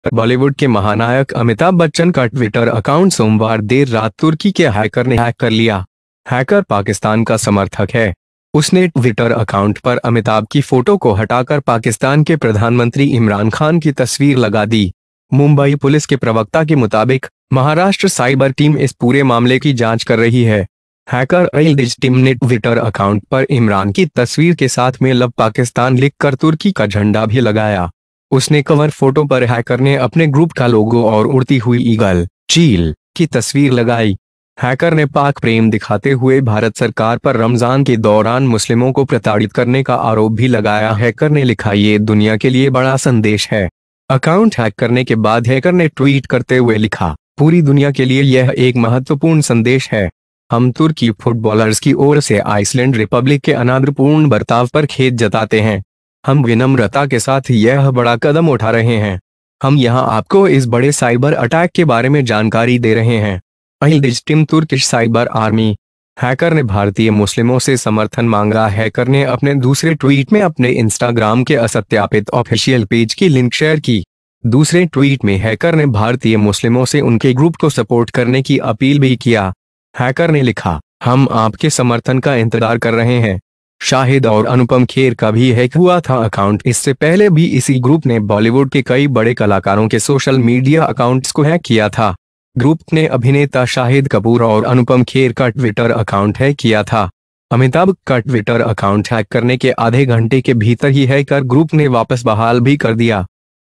तुर्की हैकर ने अमिताभ का ट्विटर अकाउंट हैक किया
Bollywood superstar Amitabh Bachchan's Twitter account hacked on Monday night by hacker of Turkey. Hacker is a supporter of Pakistan. He removed the photograph of Amitabh on Twitter account and put a picture of Pakistan Prime Minister Imran Khan. According to the Mumbai Police spokesperson, the Maharashtra Cyber ??Team is investigating this whole case. Hacker Ageidge Tim also posted a Turkish flag on the Twitter account with the picture of Imran, 'Love Pakistan'. On the cover photo, the hacker put a picture of his group and a flying eagle (the eagle). The hacker has shown the love of Pakistan, while blaming the Muslims for harassing the Muslims during Ramzan. He wrote that this is a big message for the world.
After hacking the account, Hacker tweeted, "This is an important message for the whole world. We regret the disrespectful behavior of the Iceland Republic on behalf of Turkish Footballers. We are taking this big step with humility. Here we are giving you information about this big cyber attack. The Yiddish Tim Turkish Cyber ??Army. Hacker sought support from Indian Muslims, who shared the link to the unverified official page of their Instagram, in their second tweet. In another tweet, the hacker also appealed to Indian Muslims to support their group. Hacker wrote, "We are waiting for your support." Shahid and Anupam Kher were also hacked. Earlier this group had hacked social media accounts of many Bollywood actors. The group hacked actor Shahid Kapoor and Anupam Kher's Twitter account. Hacker Group has restored itself within half an hour of hacking Amitabh's Twitter account.